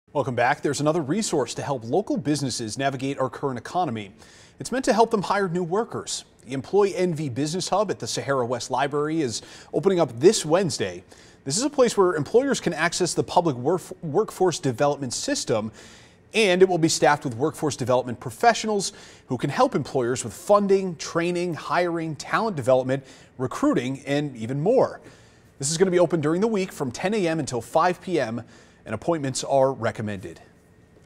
New 'Employ NV Business Hub' debuting at Sahara West Library
Times are challenging for small local businesses, but help is on the way to help them recover and grow with the debut of the newest “Employ NV Business Hub” located inside the Las Vegas-Clark County Library District’s Sahara West Library at 9600 W. Sahara Ave.